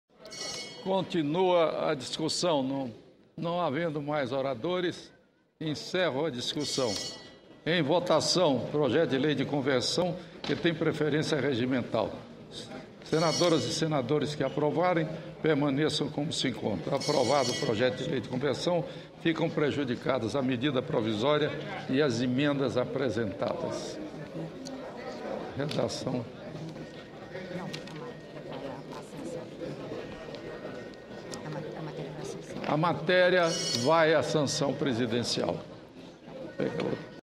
Senado aprova MP 496, que estabelece novos limites de endividamento a município-sede da Copa de 2014
24/11/2010 - Plenário Ordem do Dia - Senado aprova MP 496, que estabelece novos limites de endividamento a município-sede da Copa de 2014